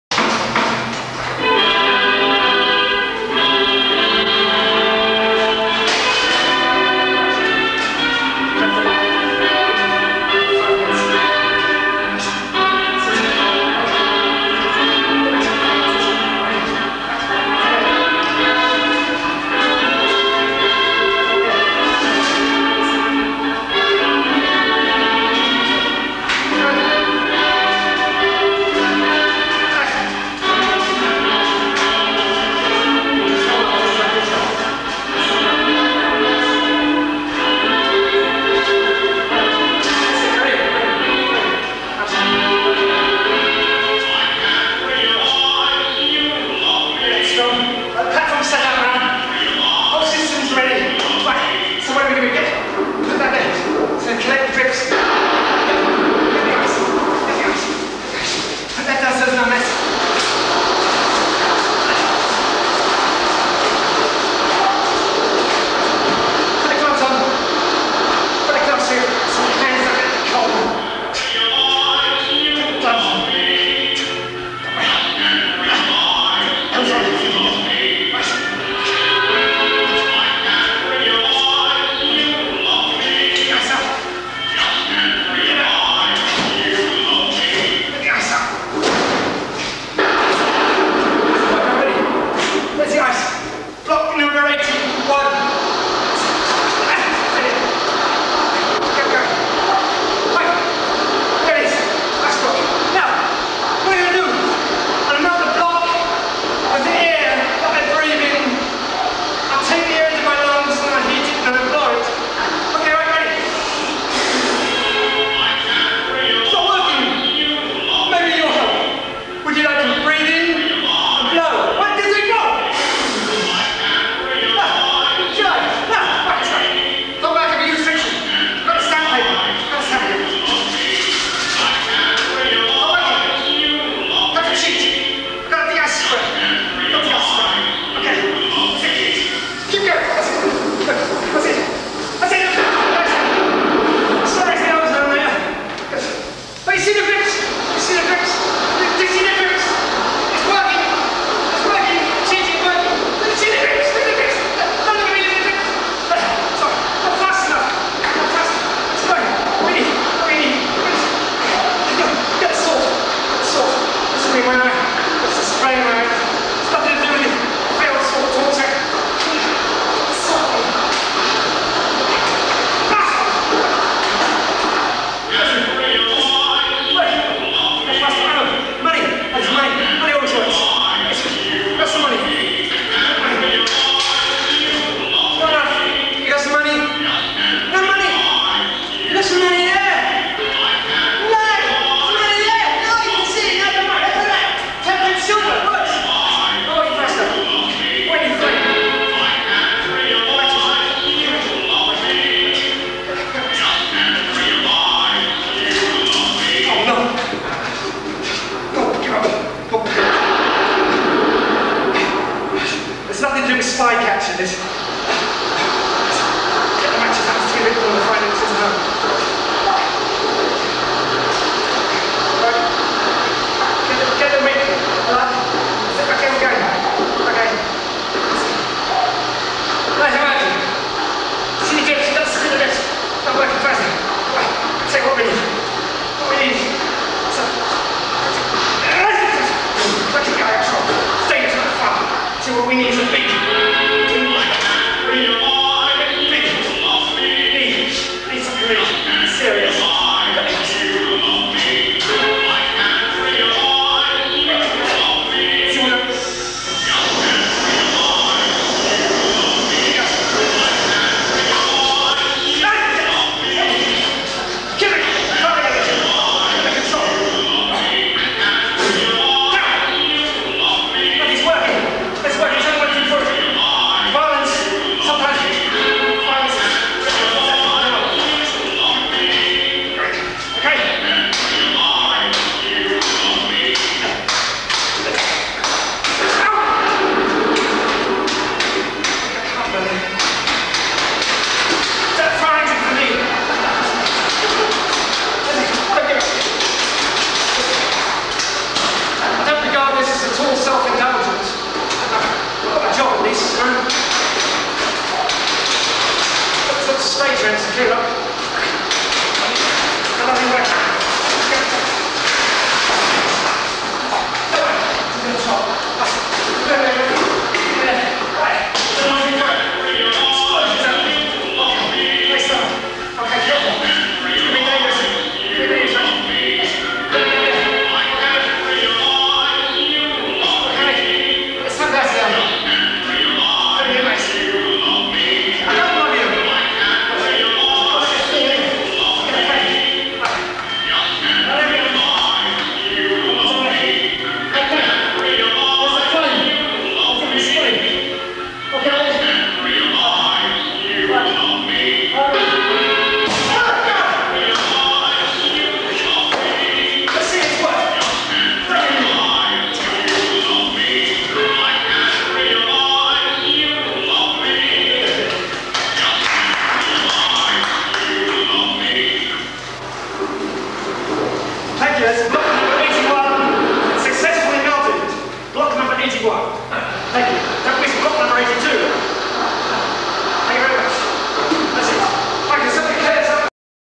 John Fleming auditions No 4: The Iceman - 1987
The Iceman - one of the more bizarre British acts of the 80s - auditions in London - 1987 - more auditions